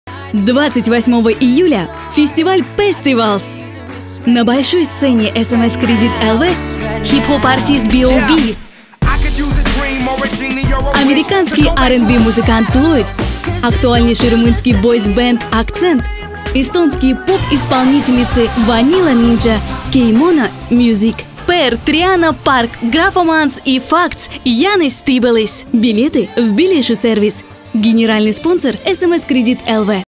Festival Pestivals - B.o.B. Lloyd Akcent Vanilla Ninja - Riga (RUSSIAN)
B.o.B., Lloyd, Akcent, Vanilla Ninja, KeyMono, Musiqq, Janis Stibelis, PeR, Triana Park, Grafomans & Fakts, DJ Gustavito.
SMScredit.lv Main stage artists list will be updated.
DJ stage
Active (sports) zone
Attractions

July 28 - Mezaparks Great stage - popular music and culture festival